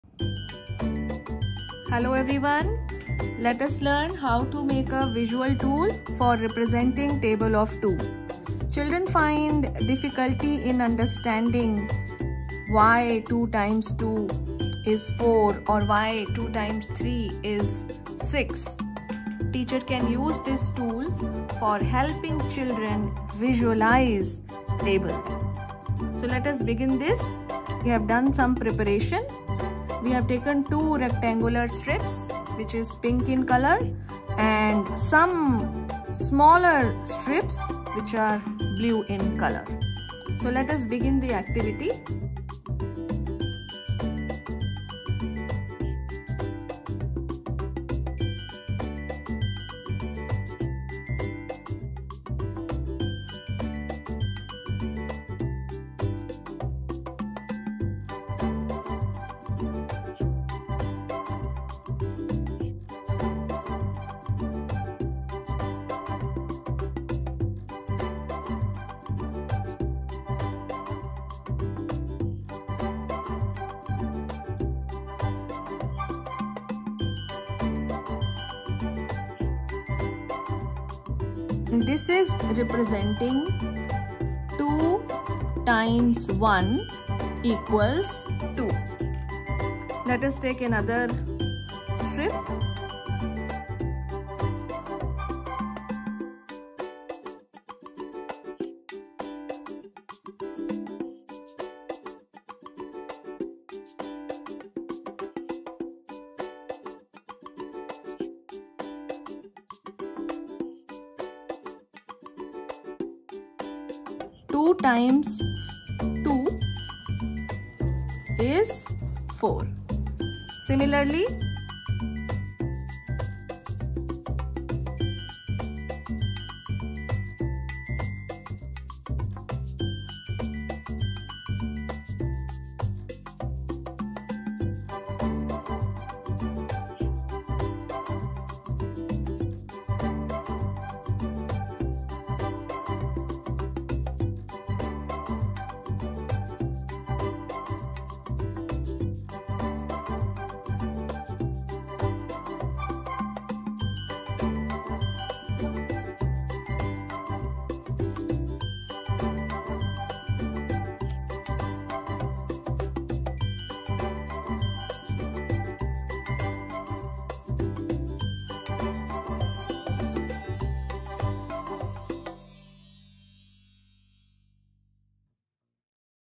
2 times table | Maths Activity | Visual Mathematics
Maths hands on activity to visualise 2 times table.